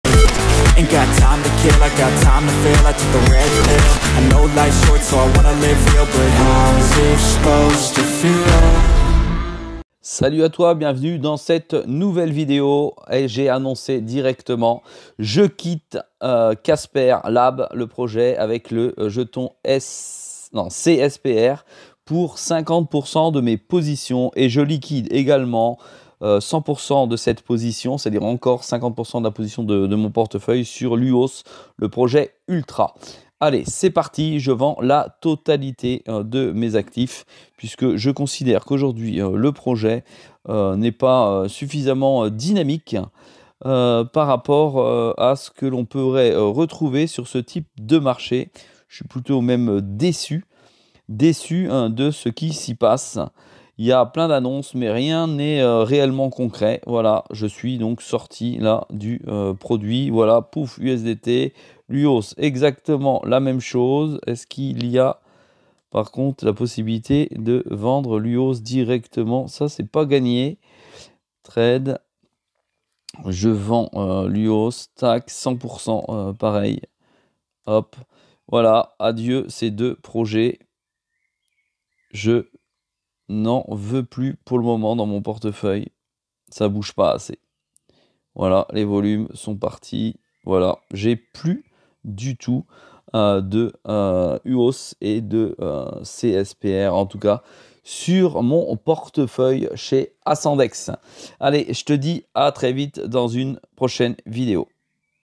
Casper et Ultra à la cave 😭 je vends mes tokens CSPR et UOS
Je sors de 50% des projets CASPER et ULTRA.
Bien entendu, les projets sont toujours très intéressants.
Néanmoins, je garde une petite position.

Bon investissement.